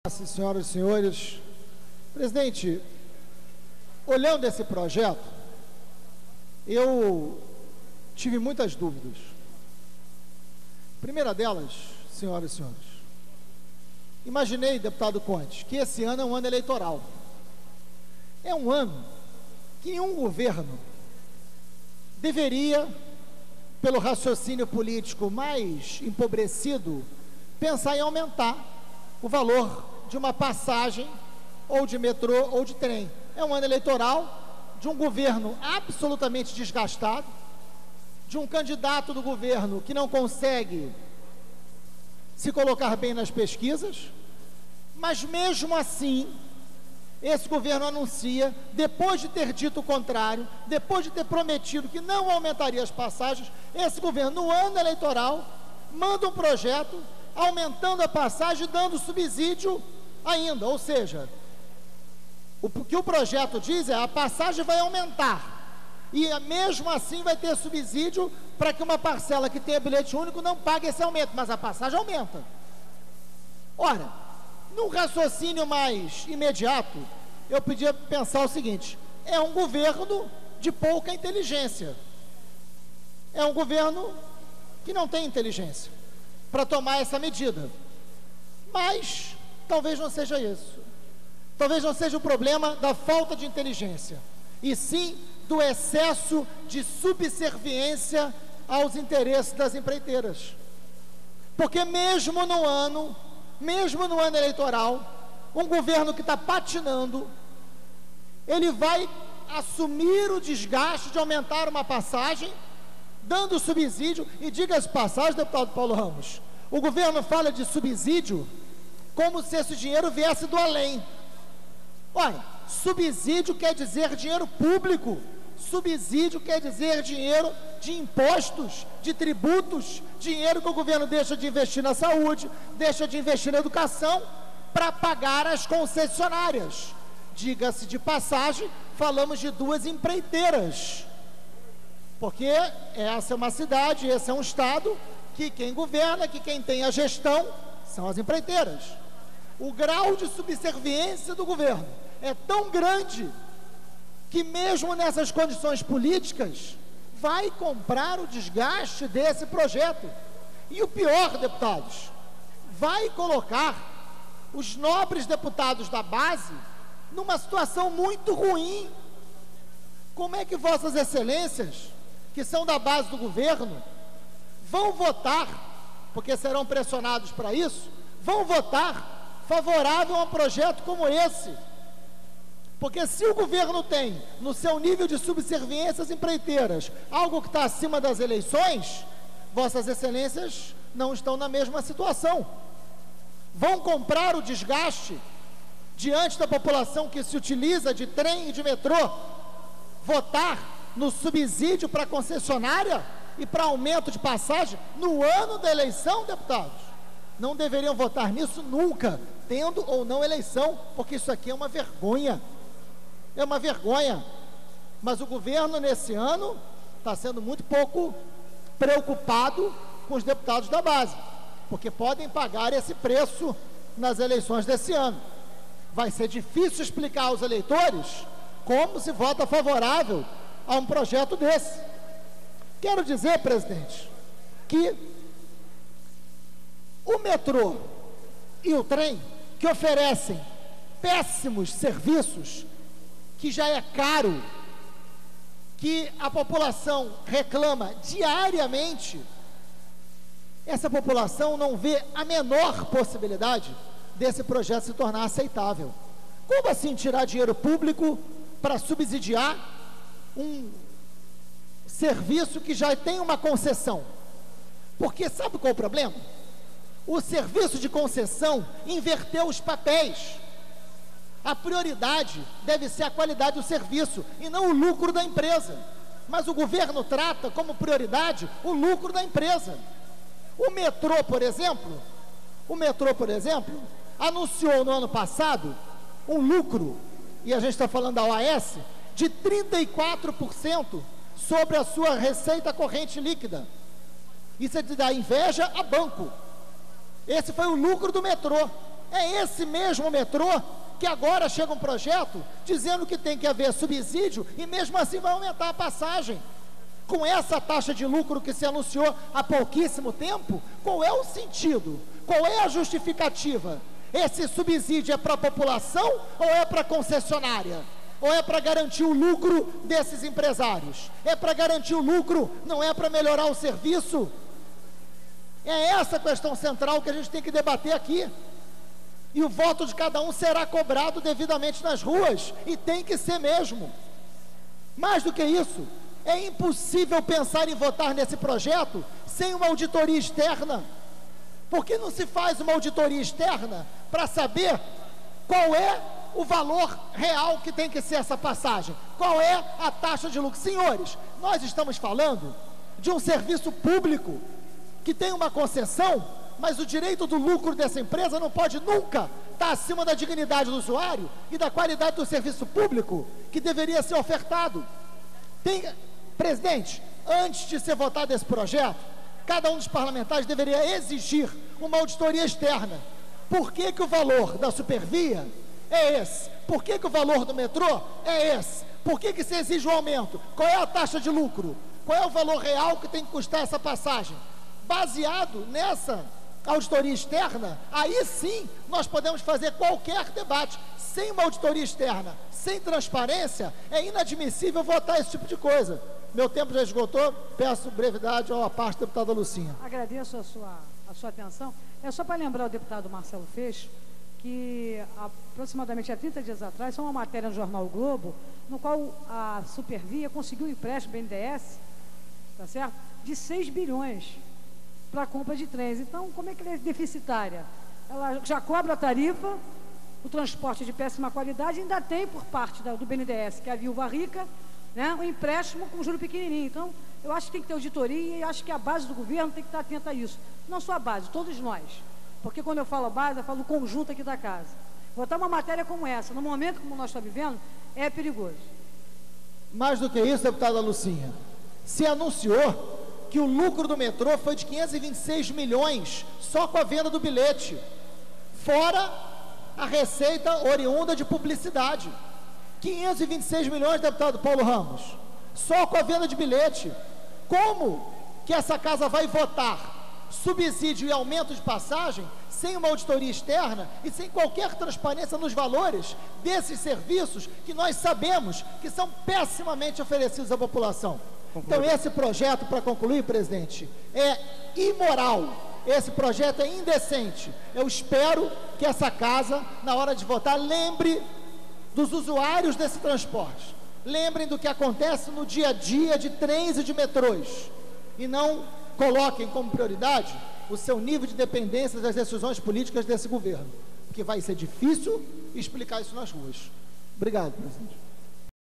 Vergonhoso aumento de trem e metrô
"Este ano é um ano eleitoral de um governo absolutamente desgastado, e este mesmo governo manda um projeto para aumentar a passagem do trem e metrô -- além de dar subsídios -, mesmo depois de já ter afirmado que não haveria reajuste. O que o projeto diz é que a passagem vai aumentar, e mesmo assim irá ter subsídio para que uma parcela que tem Bilhete Único não pague este aumento. Num raciocínio mais imediato, eu diria que é um governo de pouca inteligência. (...) Como assim tirar dinheiro público para subsidiar um serviço público que já tem uma concessão? O serviço de concessão inverteu os papeis. A prioridade deveria ser a qualidade do serviço e não o lucro das empresas. É impossível pensar em votar neste projeto sem uma auditoria externa. Por que não fazem uma auditoria externa para saber qual seria o valor real desta passagem? O direito dos lucros destas empresas não pode nunca estar acima da dignidade do usuário e da qualidade do serviço", afirmou Marcelo Freixo nesta quarta-feira (19), no plenário da Alerj.